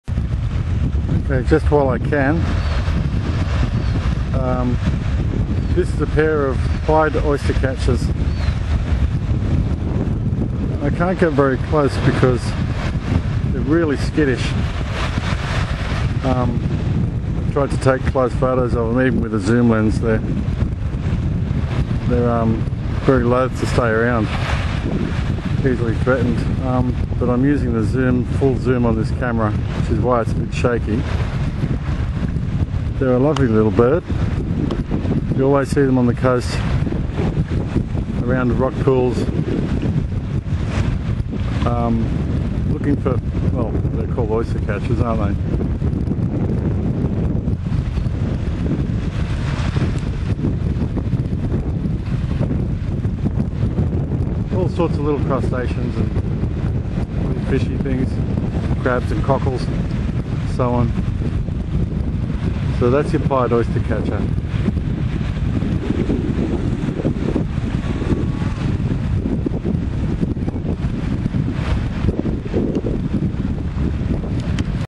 Aussie Birds Rock # 9 Pied Oyster Catcher
A pair off Pied Oyster Catchers on the shore of Botany Bay